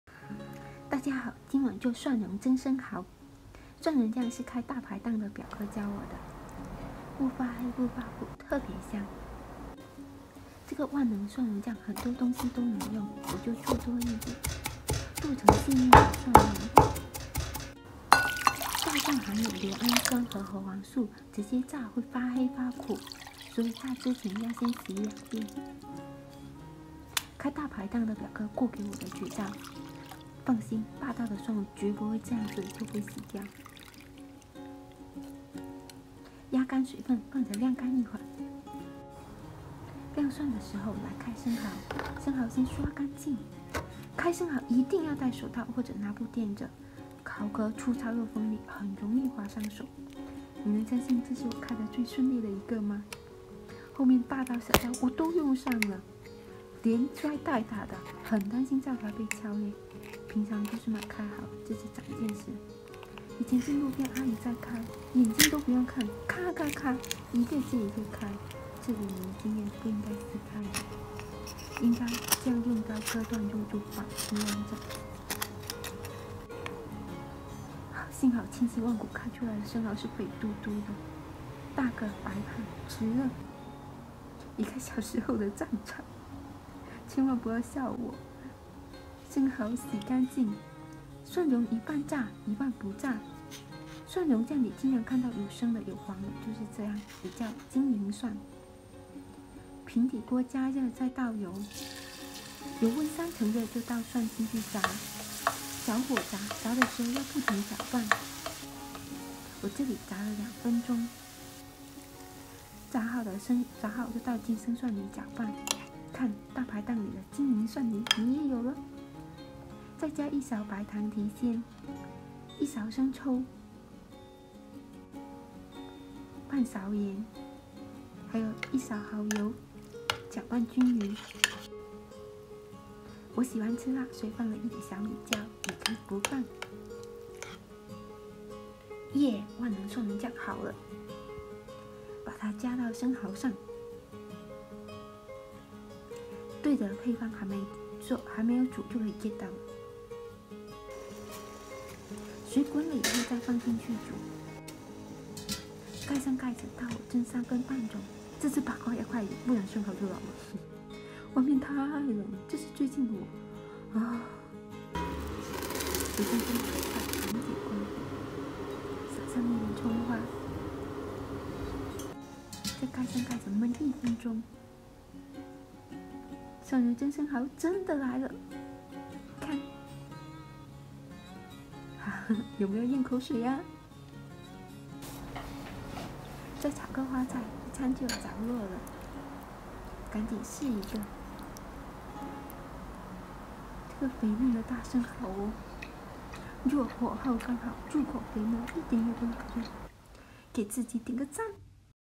愛吃生蠔的趕緊收藏啦！開大排檔的表哥教我蒸生蠔，蒸出來個個肥嫩鮮香，後悔沒有早點學
大家好，我是小魚兒，每天（除了星期一）都會上傳分享自製美食，不會擺拍，只想認真地分享一些家常菜。

**食譜：
-8個生蠔
-少許蒜蓉醬
-少許蔥花

**烹飪小貼士：
蒜蓉需要生蒜蓉和熟蒜蓉攪拌一起，做成金銀蒜。